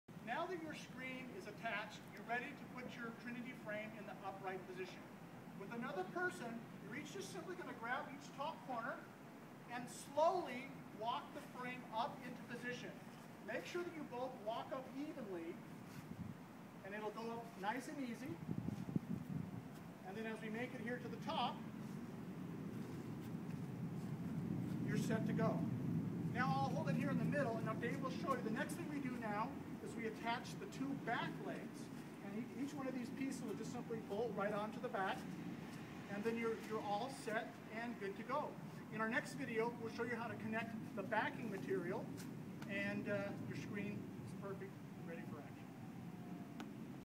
222 Manual 4 10ft Trinity Truss raising the frame
VXP's 10ft Trinity Truss. Portable, lightweight with a great industrial look. VXP VersaStretch Movie Screen. Front and rear Projetion. everythiung connects together with 1 wrench.
Video Manual 4 of 6